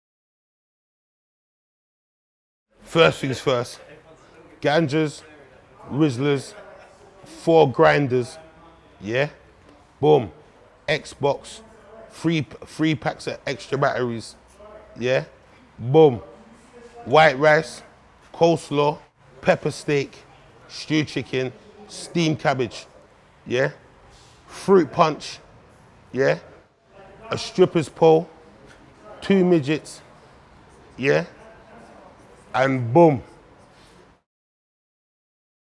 Eats & Beats: Big Narstie's Ultimate Banquet
Planning a night in? Let BDL Skank maestro Big Narstie devise your shopping list.